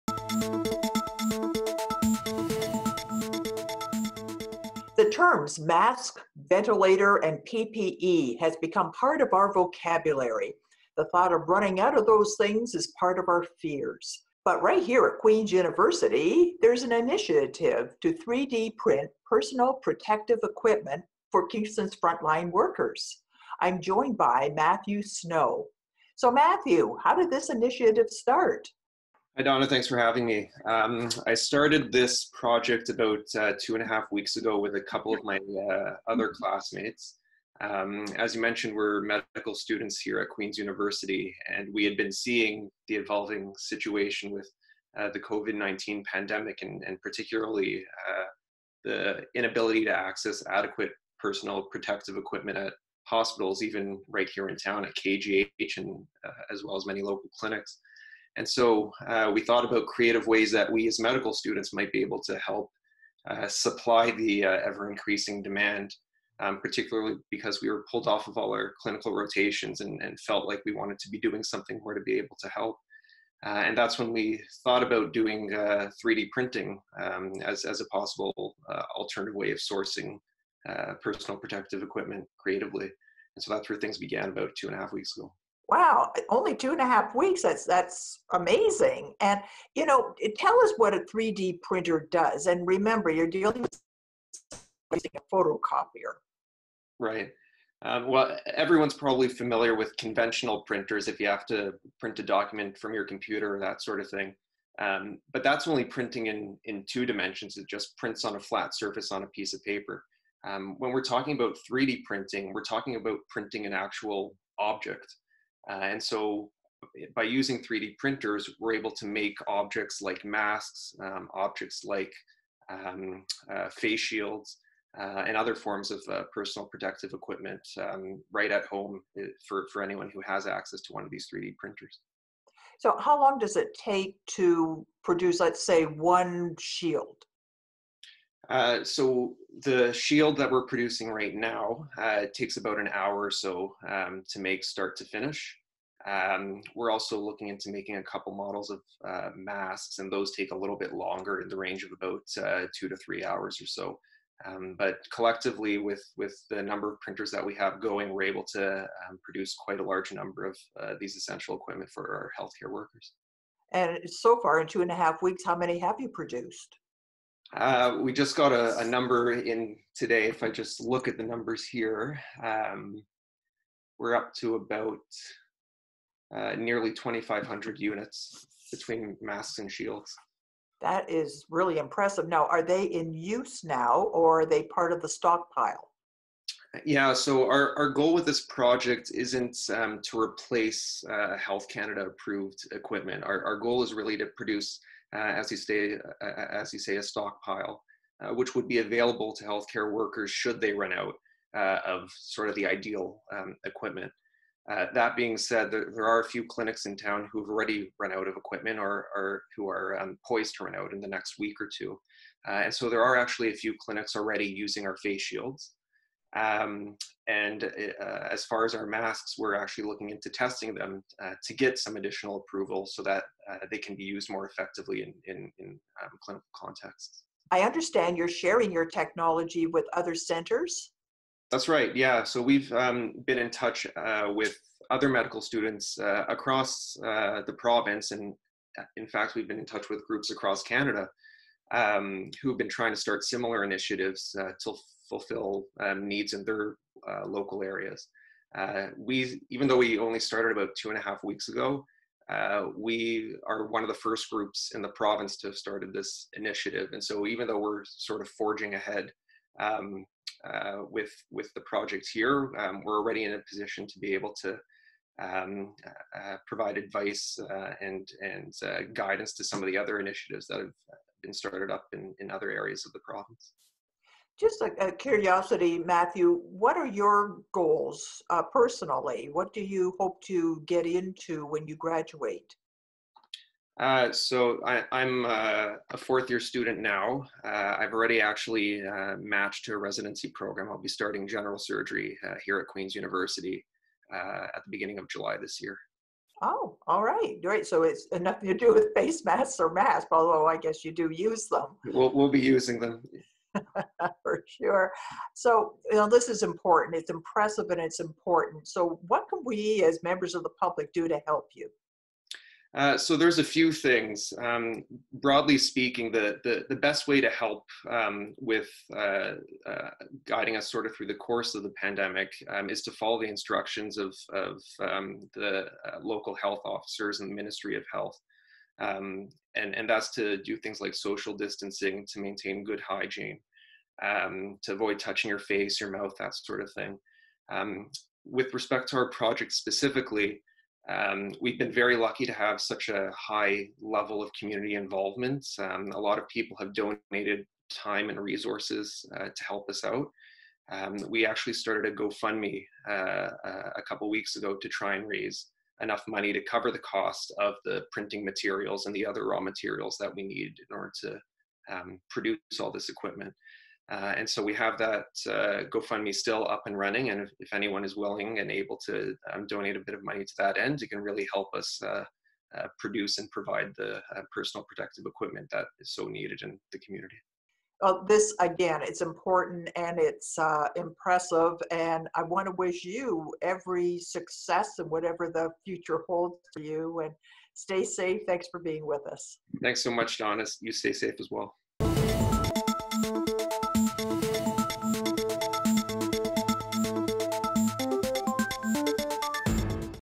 Kingston Ontario Group, 3D Printing of Face Shields for COVID-19
A Kingston area group has individuals working together to produce 3D printing of face shields and PPE (personal protective equipment) for our frontline healthcare workers in the Kingston area.